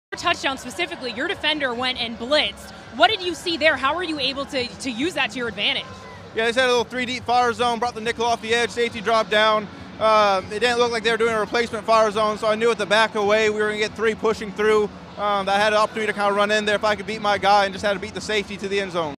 Cooper Kupp Technical answer
Cooper Kupp giving the most technical answer